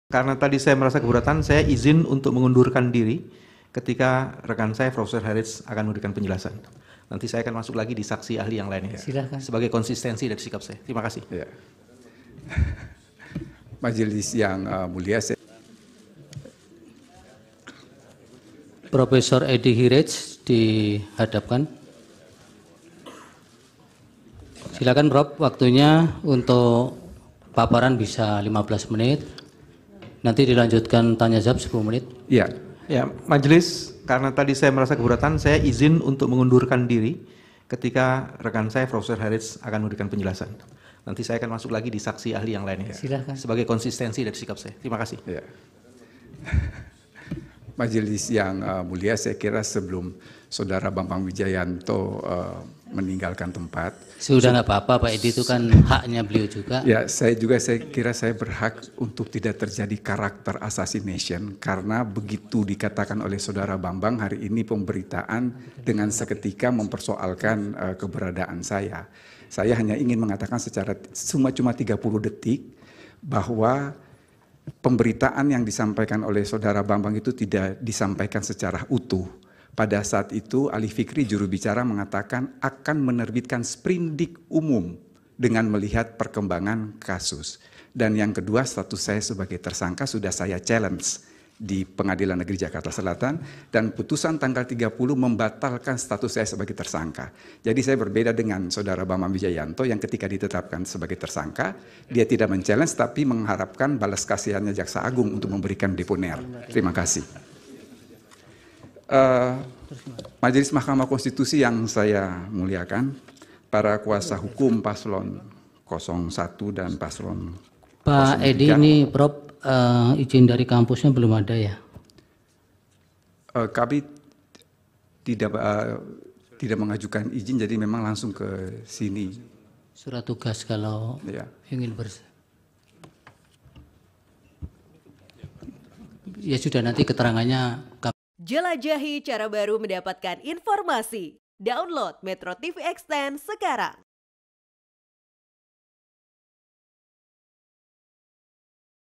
BREAKING NEWS - Panas! Eddy Hiariej VS Bambang Widjojanto di Sidang MK, Singgung Kasus Korupsi
MetroTV, Anggota tim hukum Timnas Anies-Muhaimin (AMIN), Bambang Widjojanto, mengajukan keberatan terhadap kehadiran Mantan Wakil Menkumham Edward Omar Sharif Hiariej atau Eddy Hiariej yang dihadirkan sebagai ahli oleh Pihak Prabowo-Gibran.